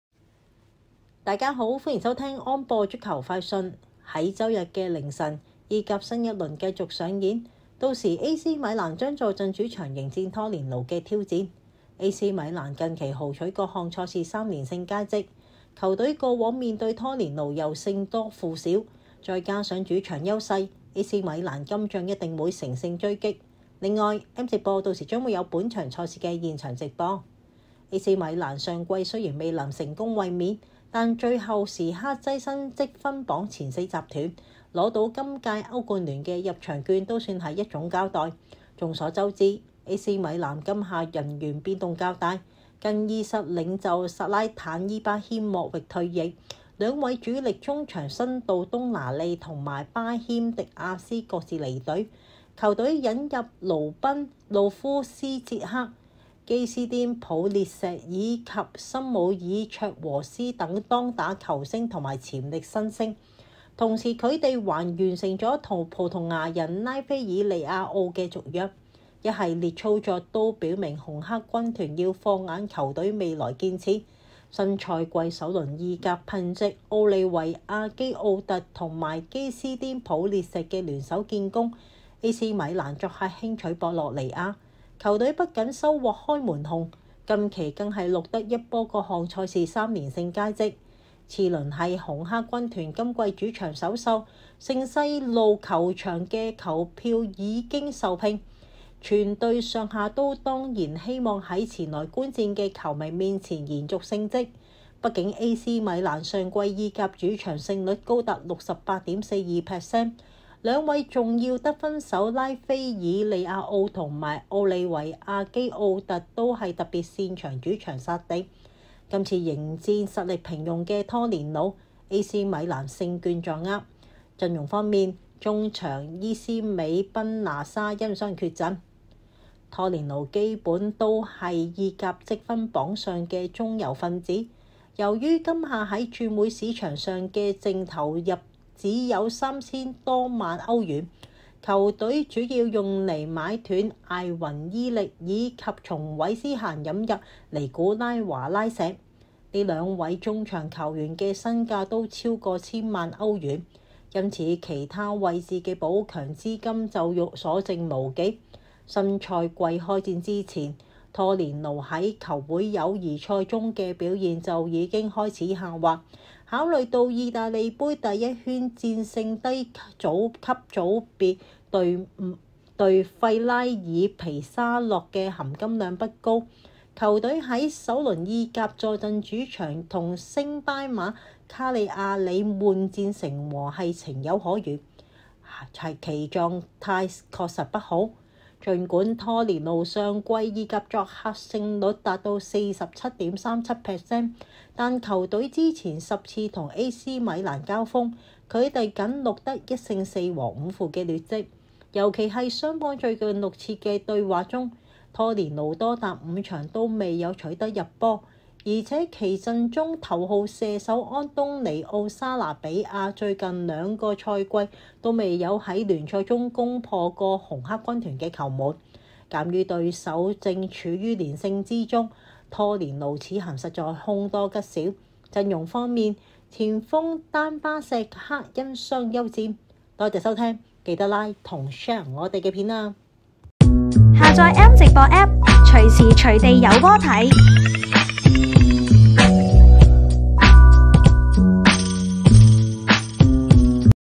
【意甲-賽前分析】2023-08-27 AC米蘭 VS 拖連奴 | AC「連」勝在望 [聲音報導: Jojo]
週日凌晨，意甲 新一輪繼續上演，屆時#AC米蘭 將坐鎮主場迎接#拖連奴 的挑戰。AC米蘭近期豪取各項賽事三連勝佳績，球隊過往面對拖連奴又勝多負少，再加上主場優勢，AC米蘭今仗一定會乘勝追擊。另外，M直播屆時將會有本場賽事的現場直播。

AC米蘭上季雖然未能成功衛冕，但最後時刻躋身積分榜前四集團，拿到今屆歐冠聯的入場券也算是一種交代。眾所周知，AC米蘭今夏人員變動較大，更衣室領袖薩拉坦.伊巴謙莫域(Zlatan Ibrahimovic)退役，兩位主力中場辛度.東拿利與巴謙.迪亞斯各自離隊，球隊引入魯賓.路夫斯捷克、基斯甸.普列錫以及森姆爾.卓禾斯等當打球星與潛力新星，同時他們還完成了與葡萄牙人拉菲爾.利亞奧的續約，一系列操作都表明紅黑軍團要放眼球隊未來建設。新賽季首輪意甲憑藉奧利維亞.基奧特與基斯甸.普列錫的聯手建功，AC米蘭作客輕取博洛尼亞，球隊不僅收獲開門紅，近期更是錄得一波各項賽事三連勝佳績。次輪是紅黑軍團今季主場首秀，聖西路球場的球票已經售罄，全隊上下也當然希望在前來觀戰的球迷面前延續勝績，畢竟AC米蘭上季意甲主場勝率高達68.42%，兩位重要得分手拉菲爾.利亞奧與奧利維亞.基奧特都是特別擅長主場殺敵，此番迎戰實力平庸的拖連奴，AC米蘭勝券在握。陣容方面，中場伊斯美.賓拿沙因傷缺陣。

拖連奴基本都是意甲積分榜上的中游份子，由於今夏在轉會市場上的淨投入只有三千多萬歐元，球隊主要用來買斷艾雲.伊歷以及從韋斯咸引入尼古拉.華拉錫，這兩位中場球員的身價都超過千萬歐元，因此其他位置的補強資金就所剩無幾。新賽季開戰之前，拖連奴在球會友誼賽中的表現就已經開始下滑，考慮到意大利盃第一圈戰勝低組別球隊費拉爾皮沙洛的含金量不高，球隊在首輪意甲坐鎮主場與升班馬卡利亞里悶戰成和是情有可原，其狀態確實不好。儘管拖連奴上季意甲作客勝率達到47.37%，但球隊此前10次與AC米蘭交鋒，他們僅錄得1勝4和5負的劣績，尤其是雙方最近6次對話中，拖連奴多達5場都沒有取得入球，而且其陣中頭號射手安東尼奧.沙拿比亞最近2個賽季都沒有在聯賽中攻破過紅黑軍團的球門。鑒於對手正處於連勝之中，拖連奴此行實在是凶多吉少。陣容方面，前鋒丹巴錫克因傷休戰。